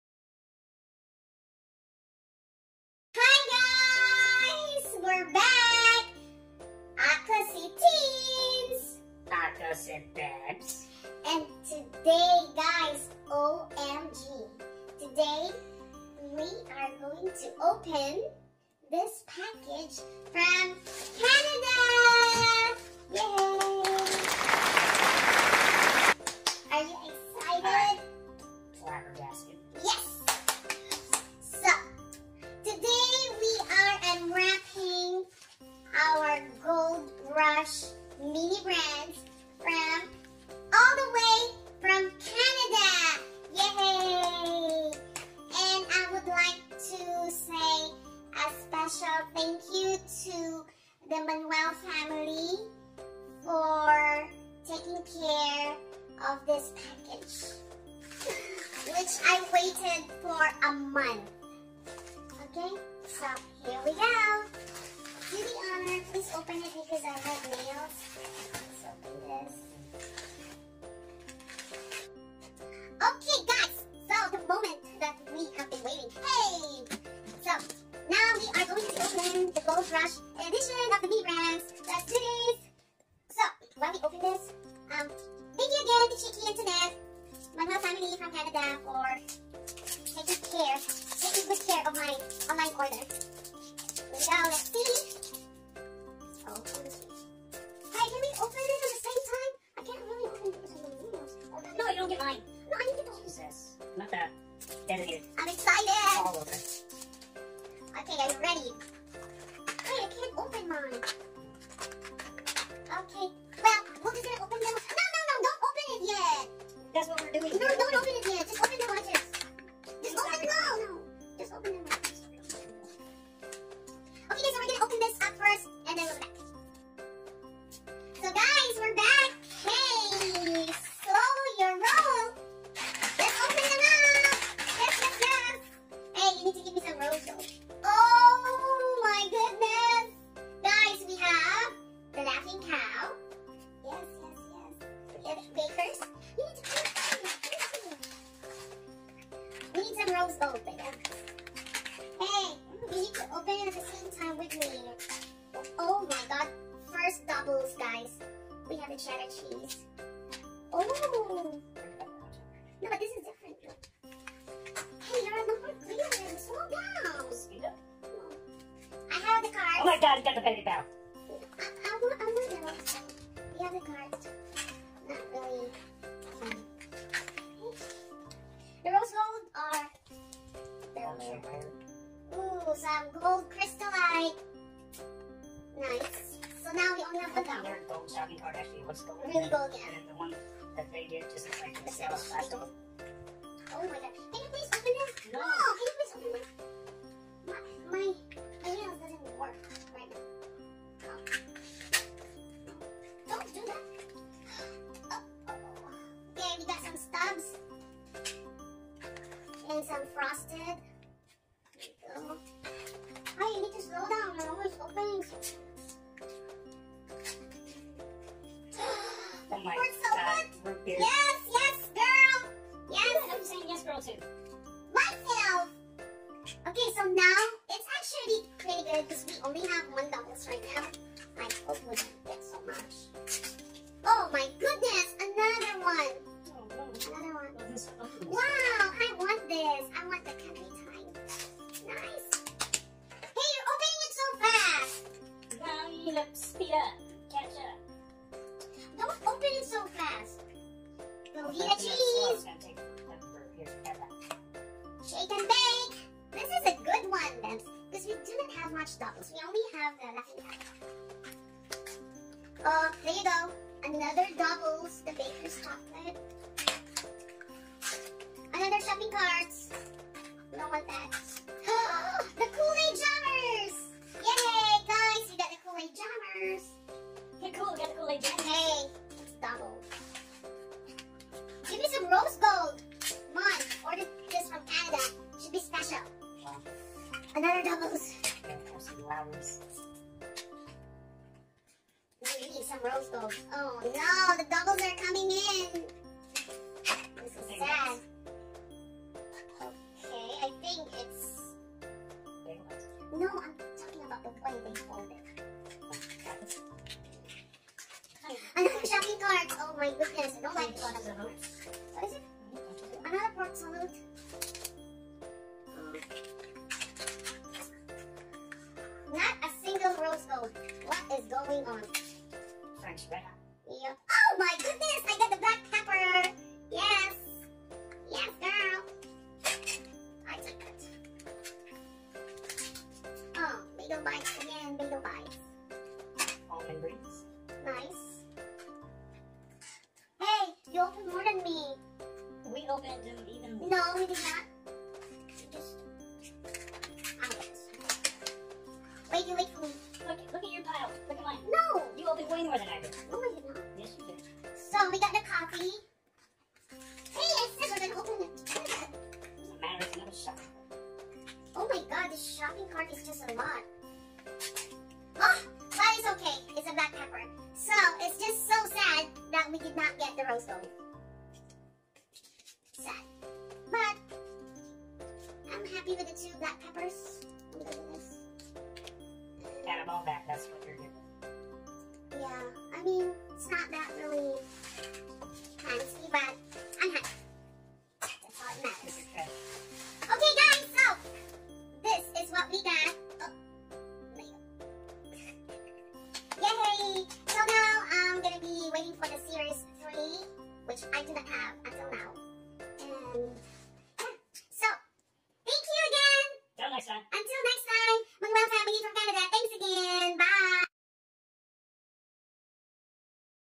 Unwrapping Gold Rush Mini Brands 5 Surprise Gold Limited Edition | Si Tins Si Bebs
Finally we were able to open the Gold Rush Mini Brands. mini brands
gold mini brands
5 surprise
mini brands gold rush
mini brands gold rush edition